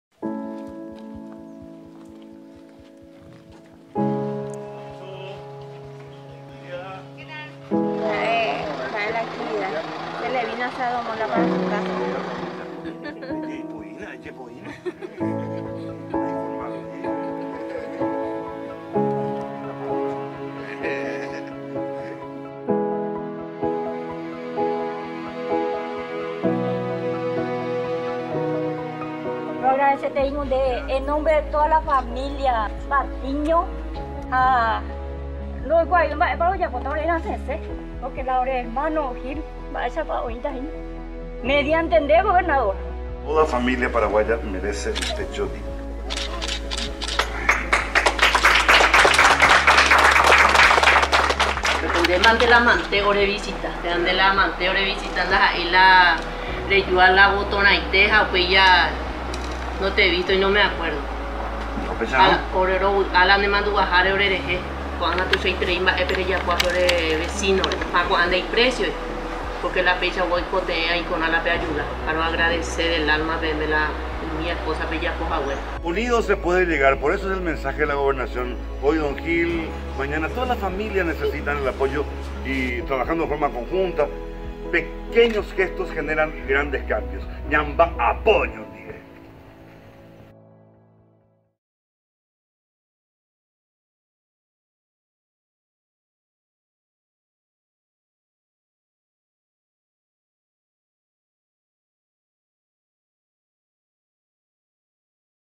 hugo javier gobernador Entrega de una vivienda a Don Gil Hilarión Molas Quintana
Entrega de una vivienda a Don Gil Hilarión Molas Quintana